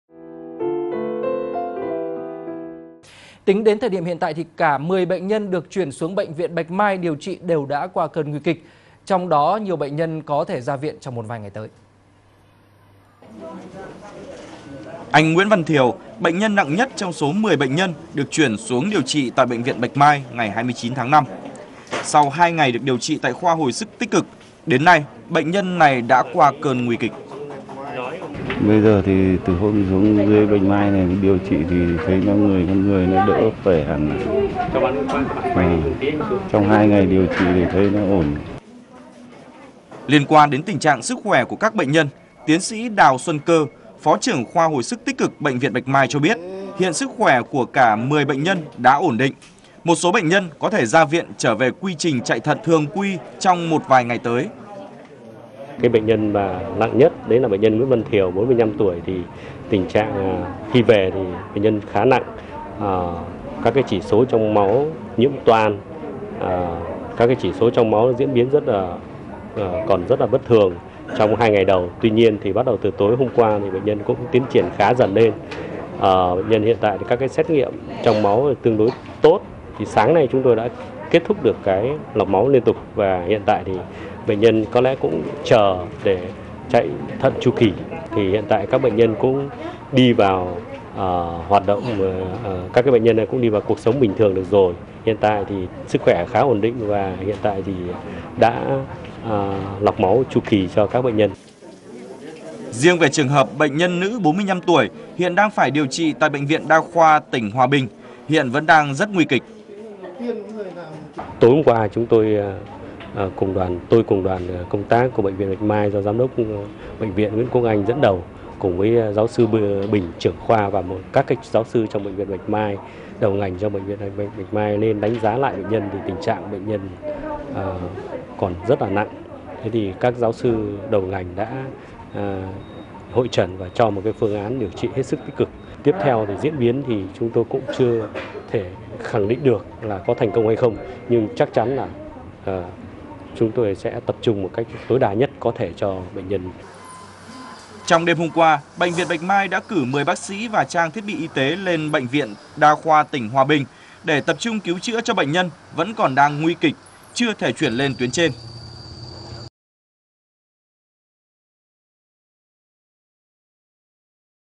VTC Tin Mới
VTC1 | Tính đến tối ngày 31/05, 10 người trong số những bệnh nhân được chuyển từ bệnh viện đa khoa Hòa Bình về bệnh viện Bạch Mai đã qua cơn nguy kịch.